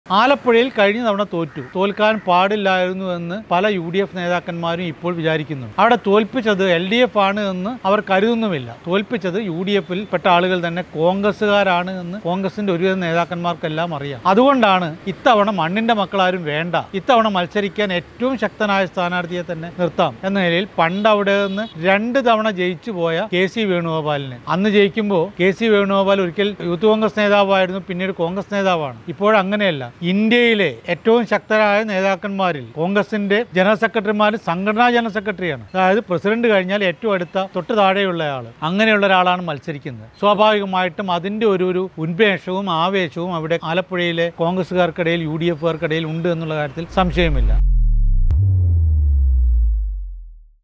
ആലപ്പുഴ കഴിഞ്ഞ തവണ കൈ വിട്ടത് എന്തുകൊണ്ട് ? | എ സജീവന്‍ | Muziriz Post
for authentic Indian reportage.

You can also follow MUZIRIZ POST's social media platforms and engage with us.